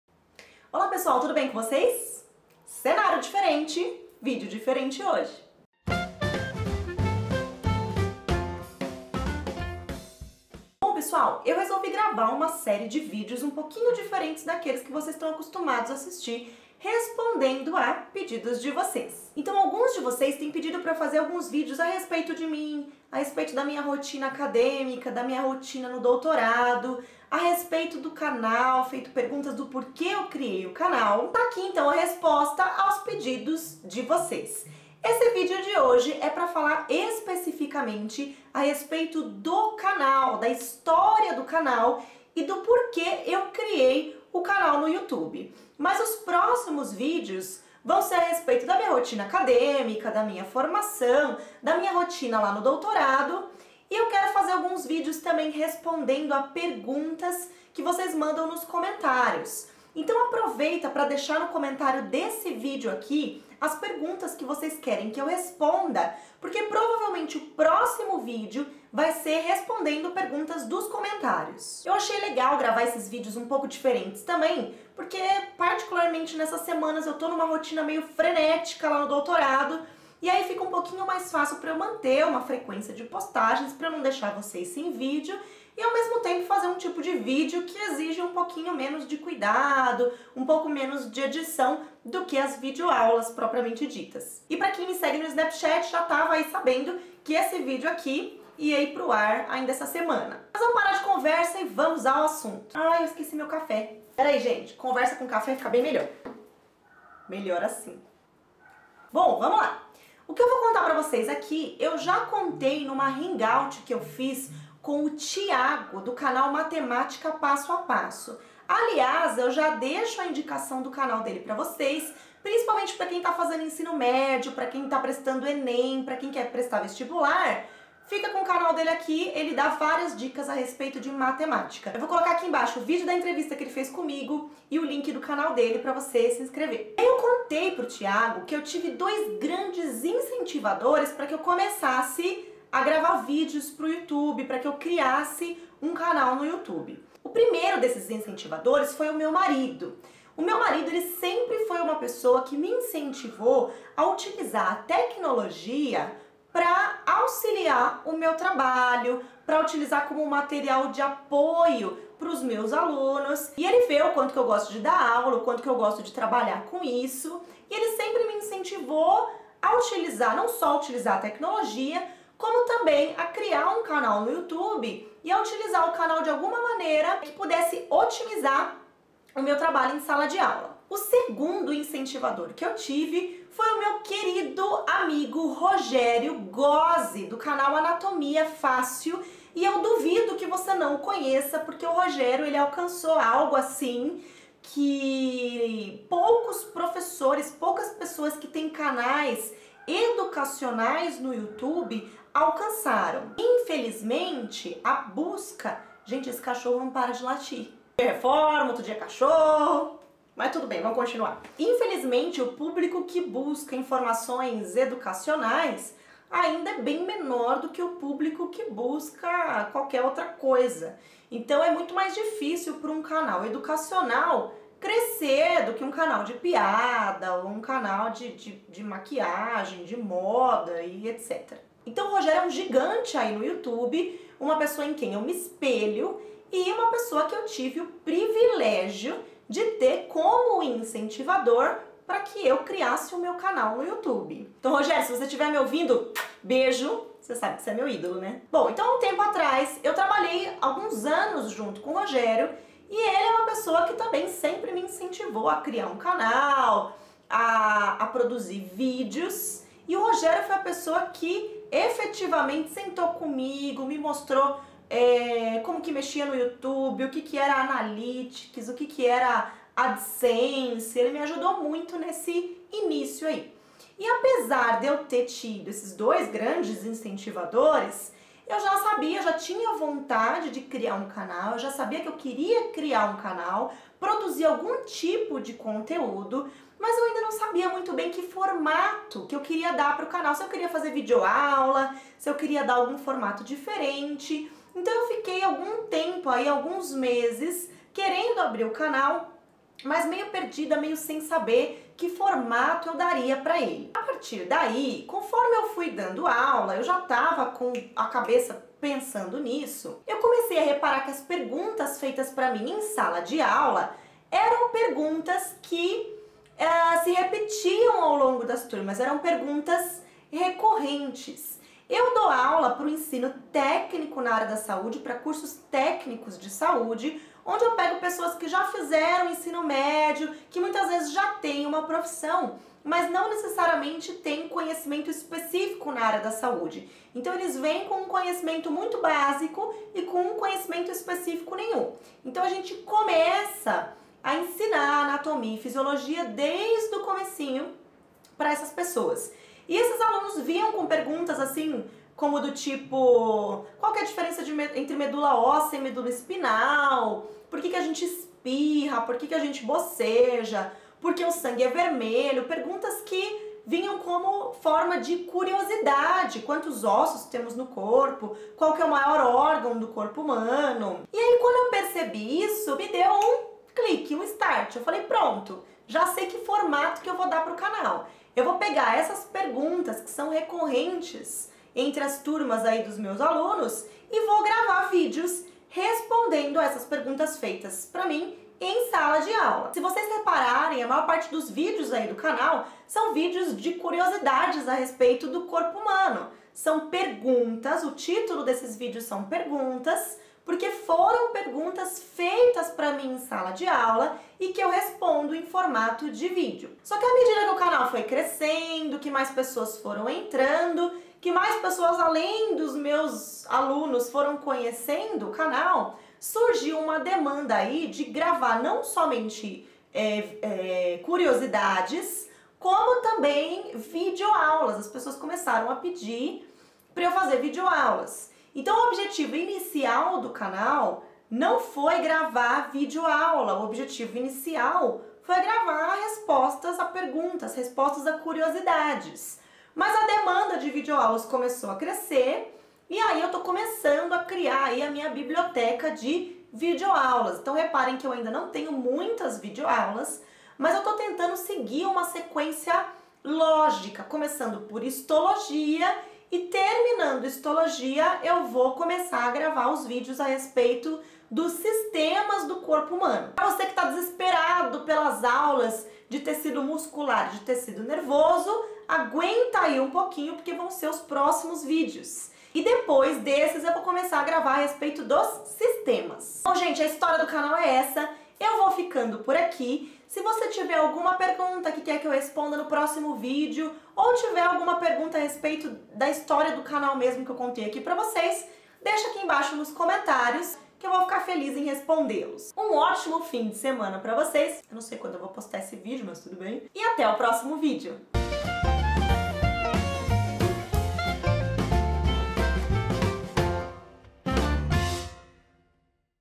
História do canal! | Anatomia e etc.
Neste vídeo eu explico quais foram as minhas motivações para criar o canal no youtube e como tive a idéia do formato do canal! Espero que gostem!